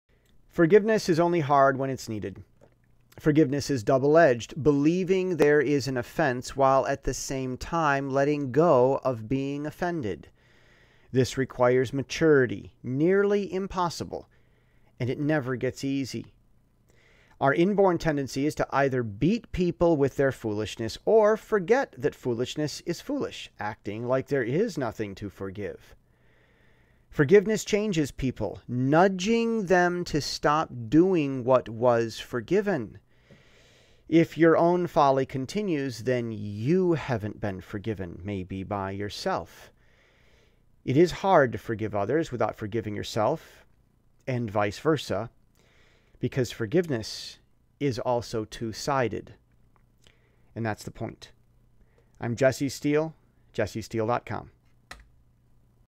Four Parts of Forgiveness | The Point
Forgiveness is only hard when it's needed. Forgiveness is double-edged: believing there is an offense while at the same time letting go of being offended. This requires maturity, nearly impossible; and it never gets easy. Our in-born tendency is to either beat people with their foolishness or forget that foolishness is foolish, acting like there is nothing to forgive.

Forgiveness changes people, nudging them to stop doing what what forgiven. If your own folly continues, then you haven't been forgiven, maybe by yourself. It is hard to forgive others without forgiving yourself, and vice versa, because forgiveness is also two-sided.